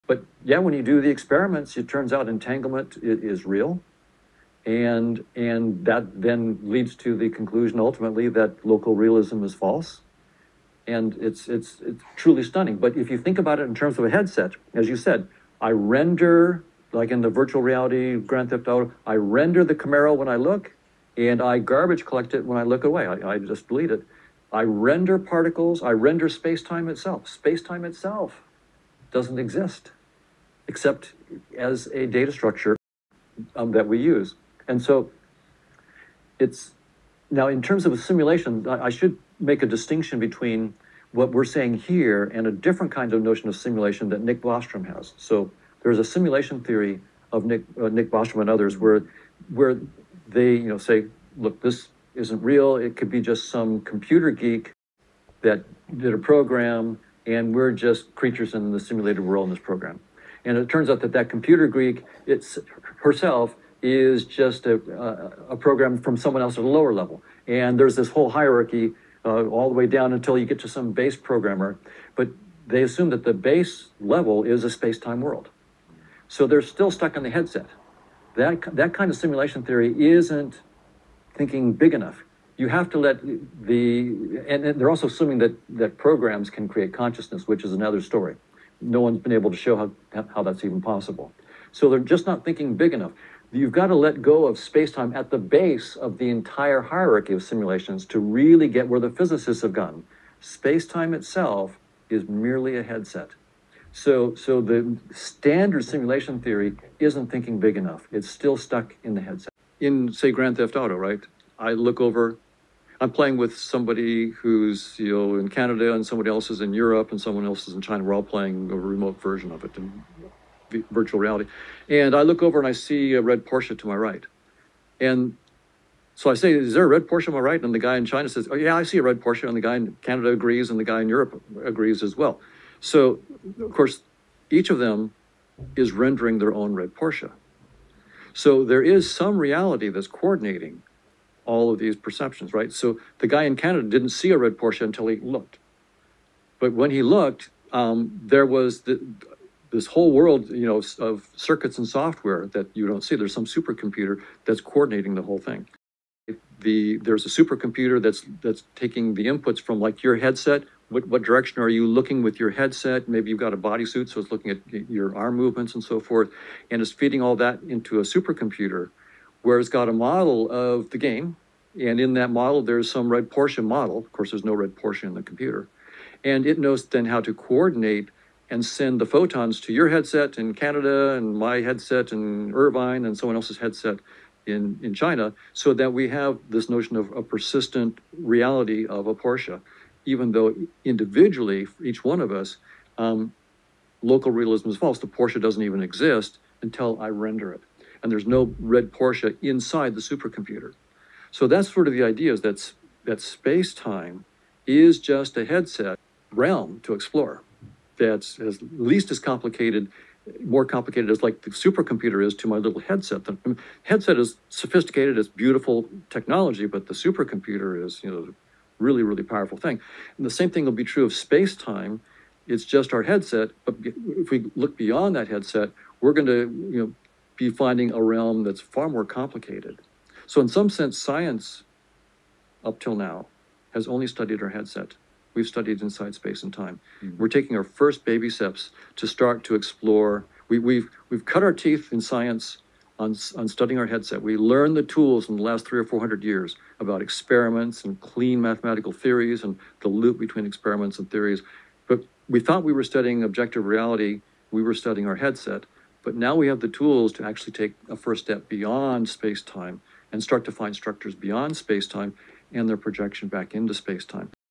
The True Nature of Reality: Donald Hoffman
Renowned cognitive scientist Donald Hoffman delves into the fascinating concept of reality as an illusion.